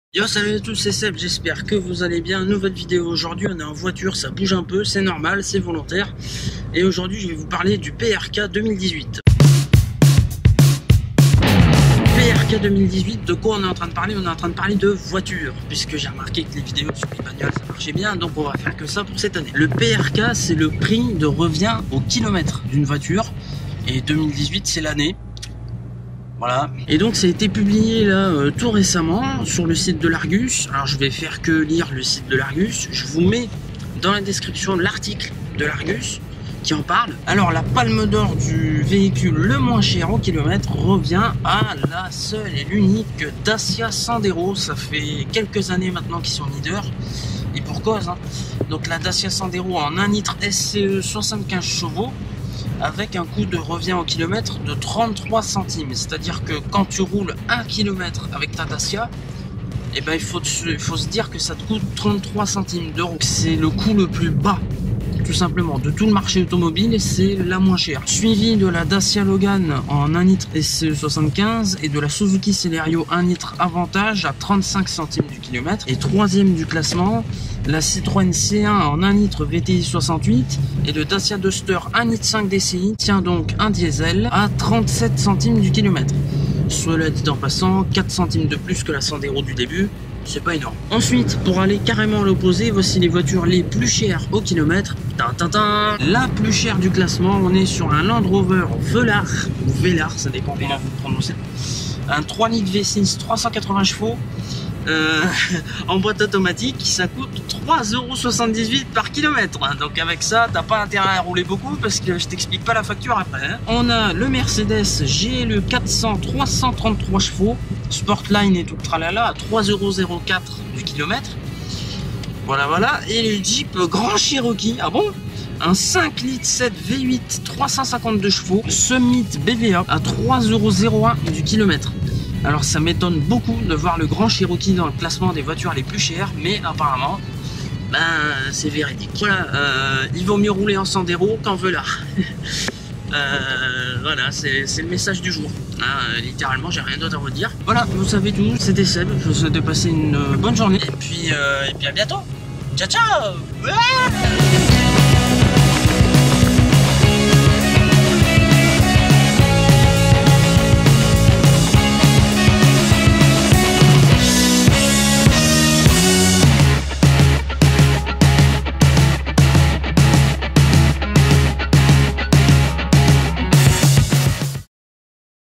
LA VOITURE LA MOINS CHERE AU KILOMETRE EST...
Le nouveau classement PRK 2018 vient d'être publié par l'argus et... c'est une grosse surprise...